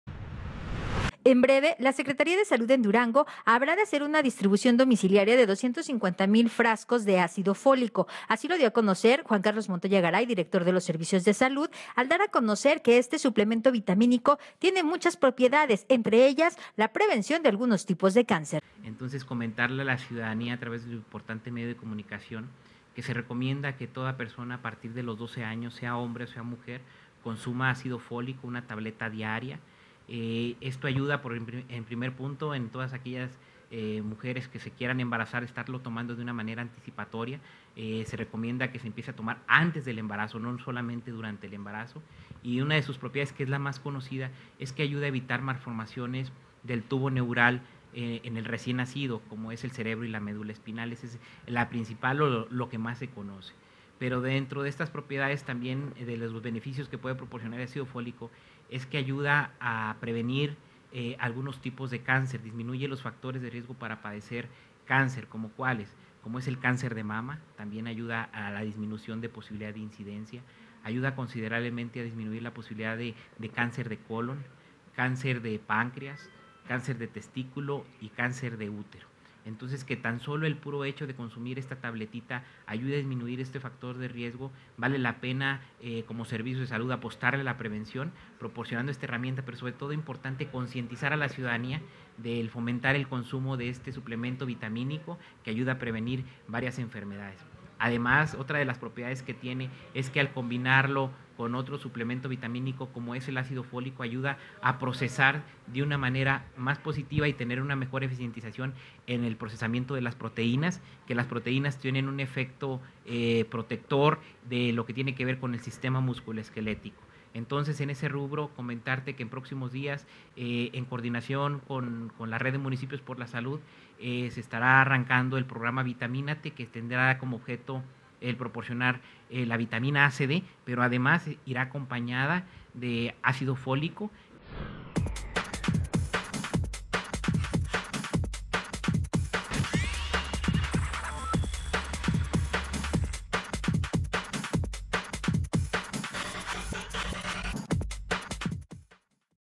Ácido fólico ayuda en prevención de cáncer
El ácido fólico tiene múltiples propiedades de protección y sobre todo para ayuda de la conservación de la salud del ser humano.Dentro de sus beneficios está el que ayuda a prevenir algunos tipos de cáncer ya que disminuye los factores de riesgo para padecer cáncer de mama, de colon, páncreas, testículo y útero, afirmó Juan Carlos Montoya Garay, director de los servicios de salud, al dar a conocer que en breve iniciarán una campaña de  distribución de este suplemento.